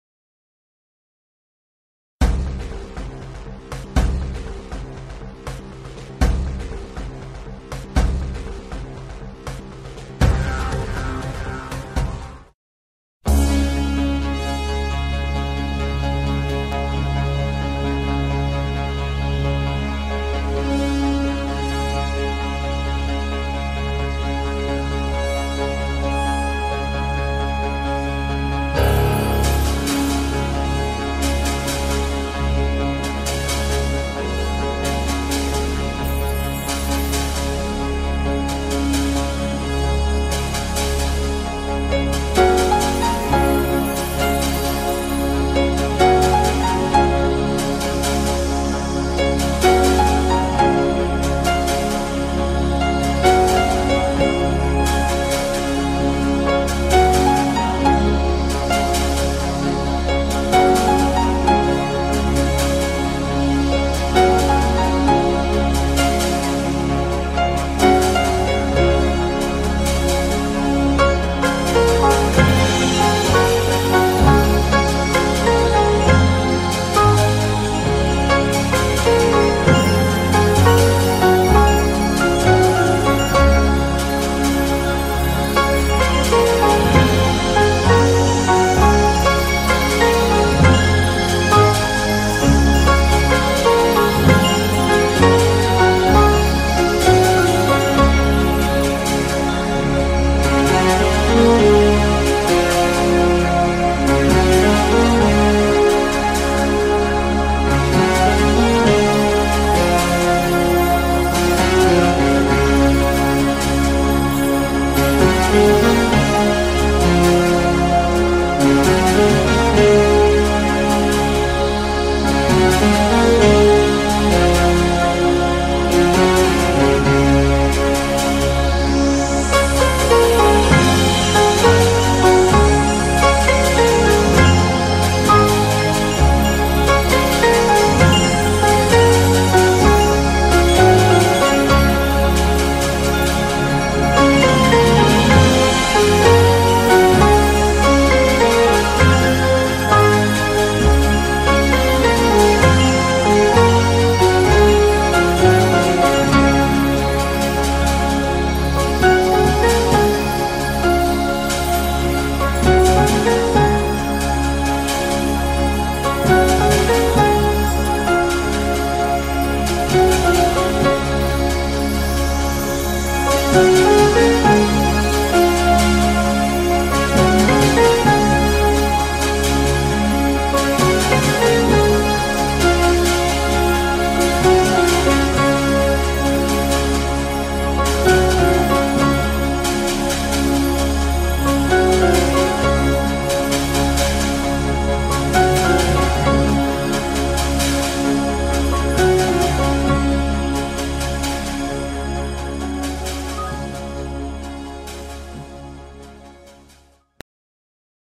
Parakramabahu National School Colours Night 2015
Background song of the colors night 2015. Interesting and very important day to sports mans.